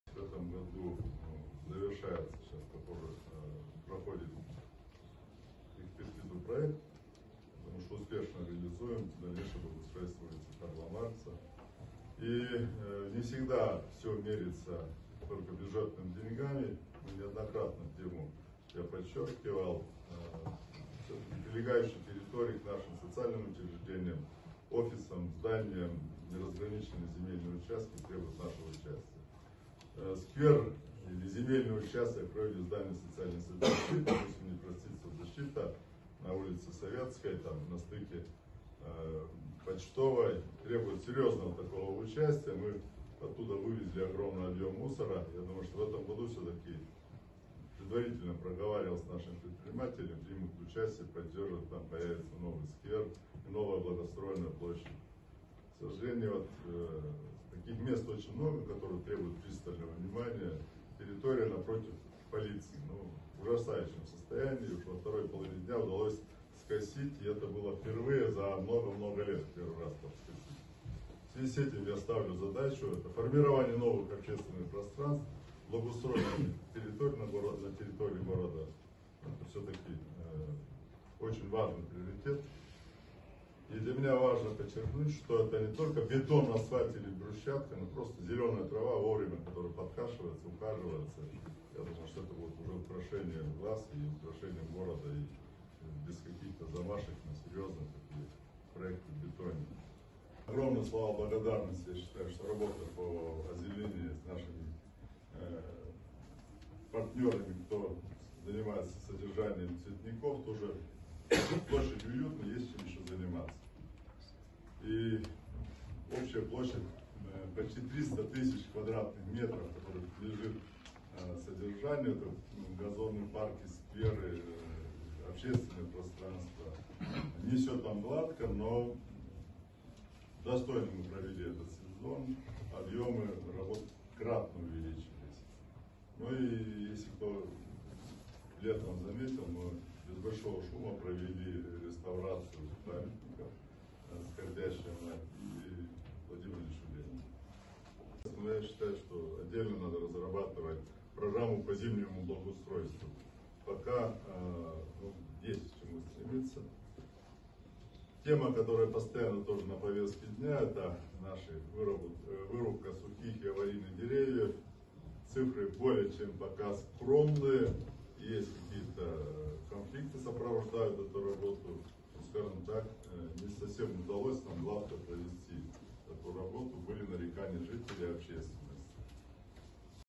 Владимир Саар
Глава города Назарово про благоустройство во время отчета